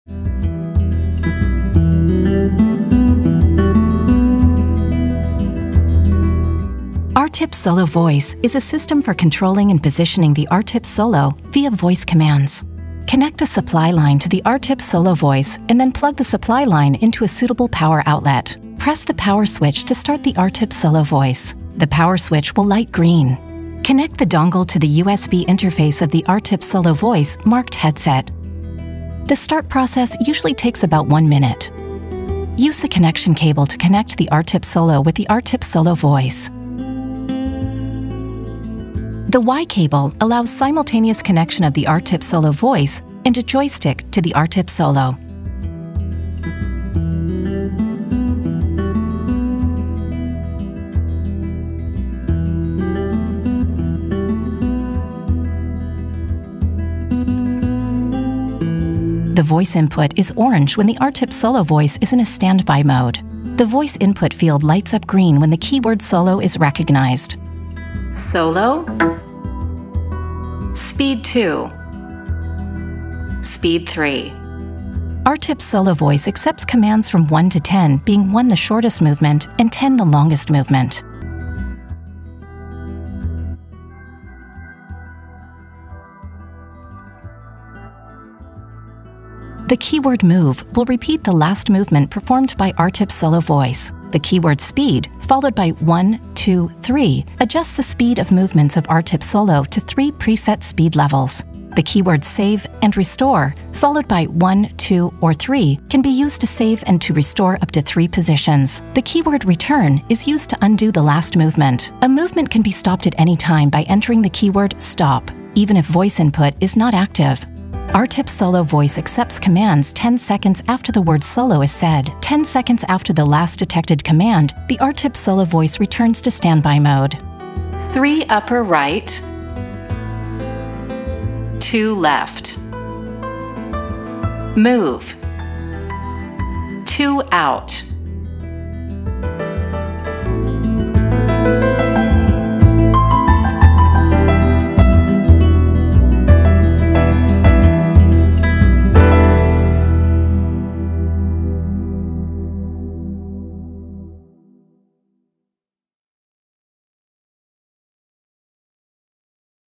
ARTip® SOLO – Hands-free Guidance with ARTip® SOLO Voice
Did you know that you can control your ARTip® SOLO using your voice? ARTip® SOLO Voice is a system for controlling and positioning the ARTip® SOLO via voice commands. In this short video, you will discover the intuitive voice commands, from movement adjustments with "move" and speed customization using "speed 1-2-3" to saving and restoring positions with "save" and "restore. Elevate your surgical experience with cutting-edge voice-guided precision!